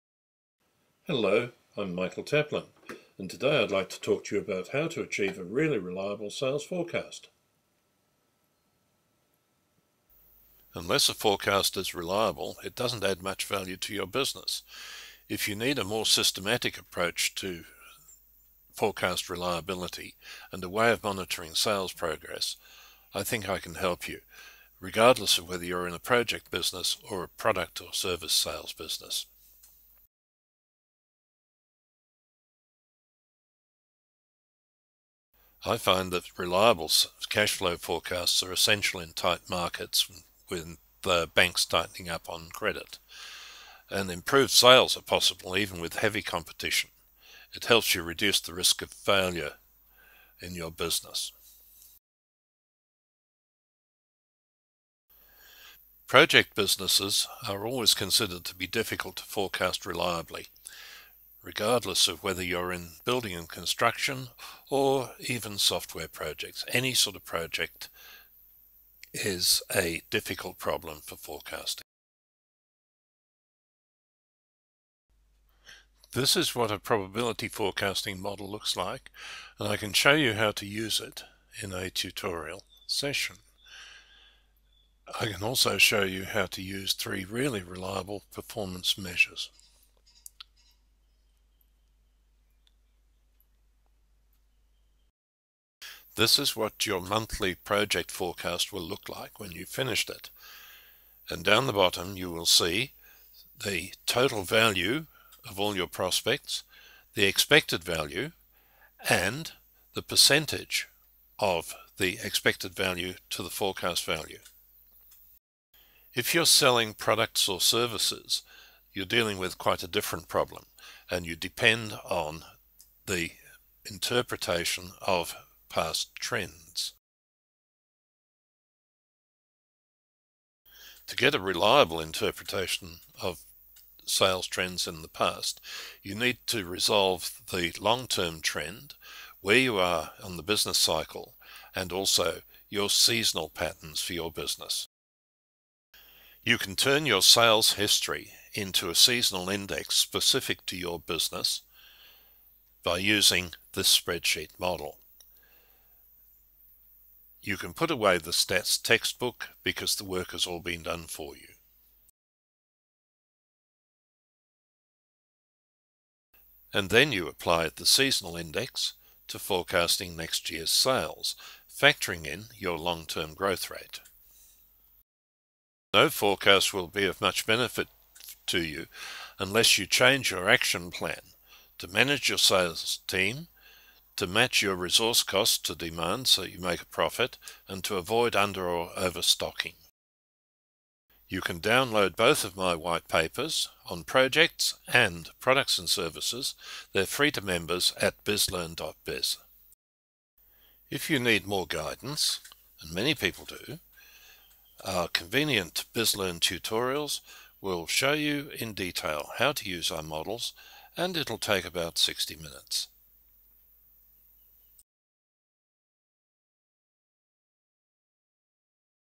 Really Reliable Sales Forecasting
Michael Taplin explains how to improve Sales Forecast reliability for projects and products or service businesses using his XL models.